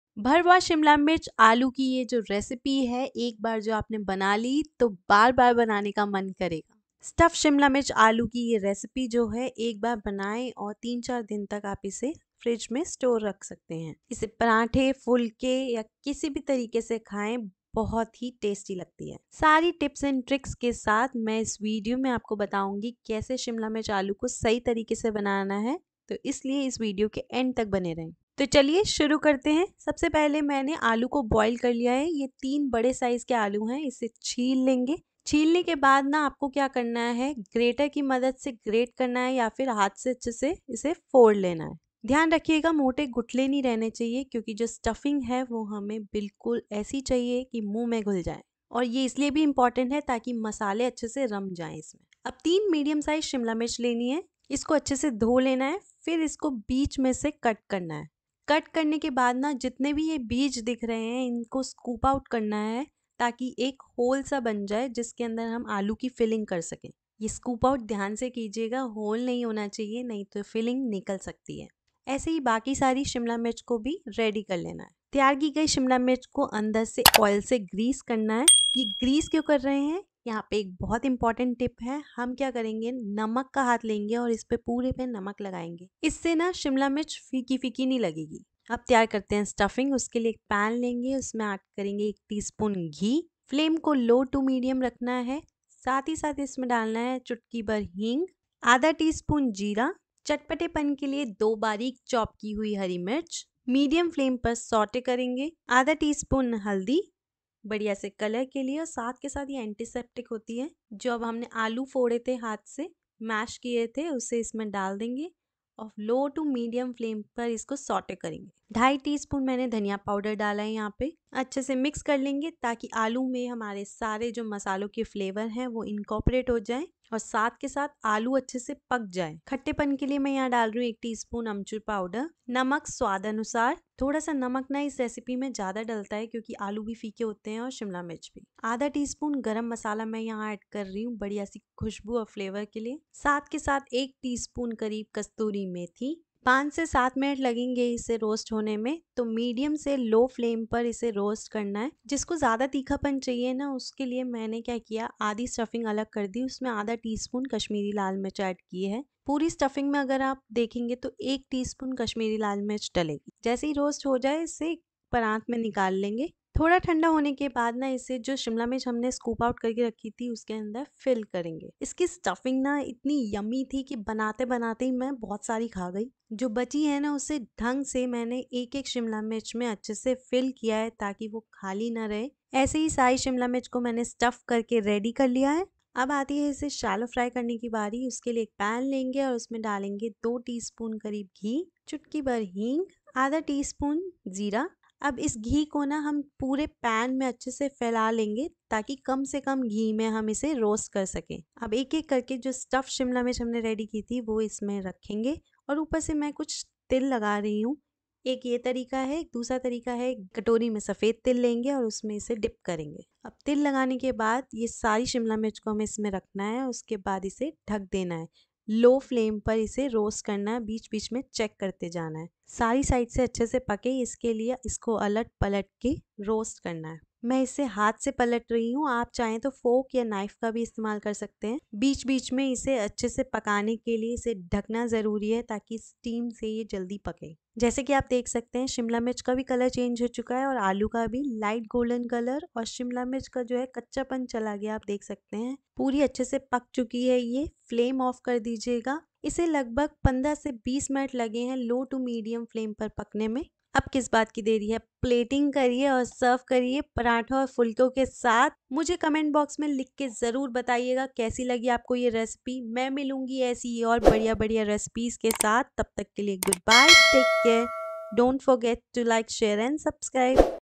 Stuffed Capsicum Recipe | भरवां शिमलामिर्च | भरवा शिमला मिर्च आलू की टेस्टी सब्ज़ी
ऐसे भरवां शिमला मिर्च बनाओगे तो उंगलियां चाटते रह जाओगे